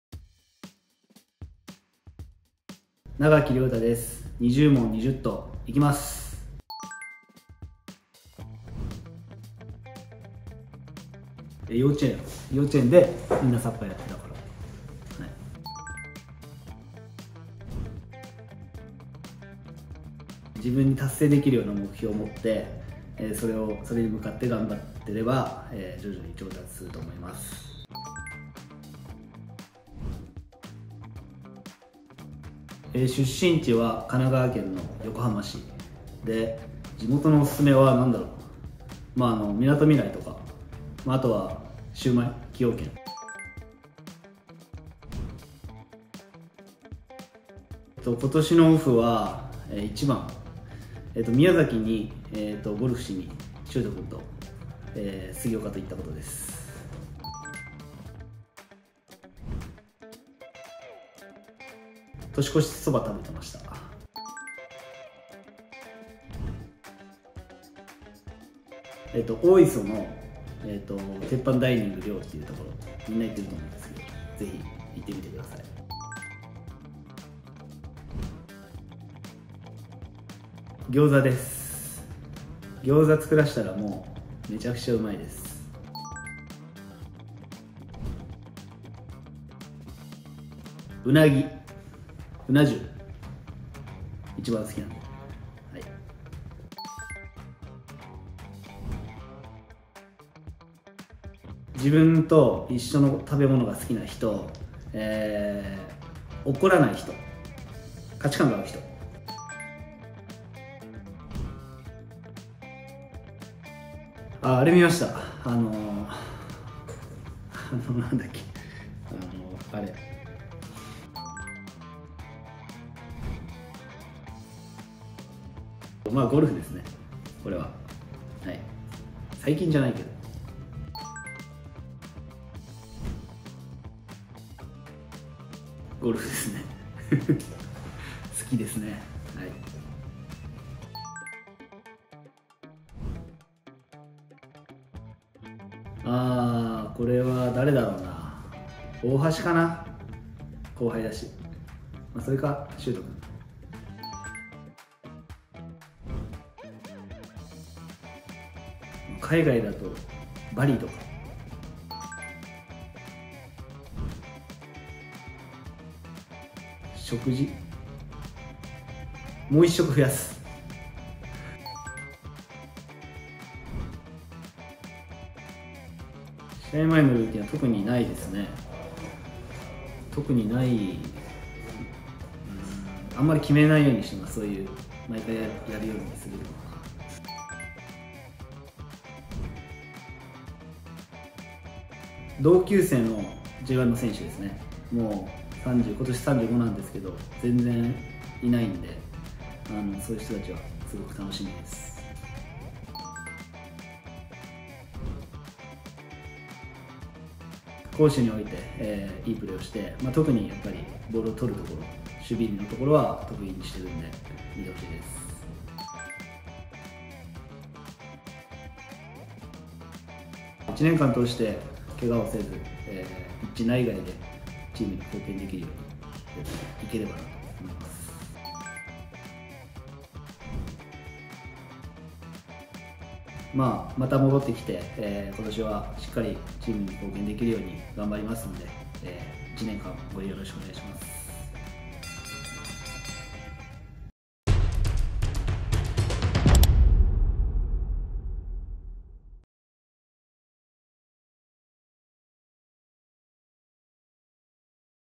2023Jリーグ開幕企画！永木亮太 選手 【全選手に聞く20問20答】Presented by リップルコミュニティ
全選手随時更新！気になる20個の質問にそれぞれ10秒で答え、選手たちの個性を知ってもらう企画です。
今回は永木亮太選手！